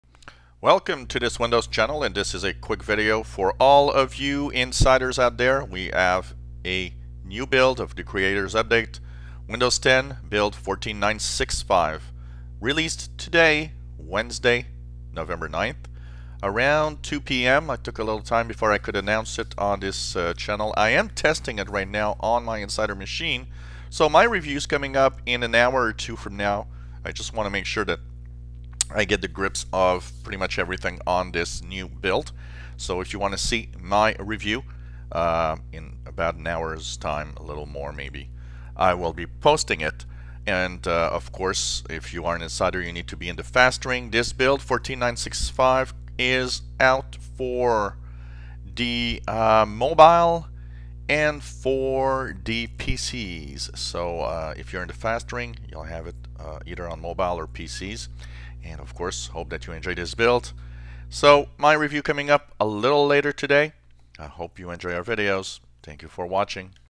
Windows 10 Creators Update Build 14965 Released November 9th 2016
A new Build of Windows 10 was released today, my review is coming up soon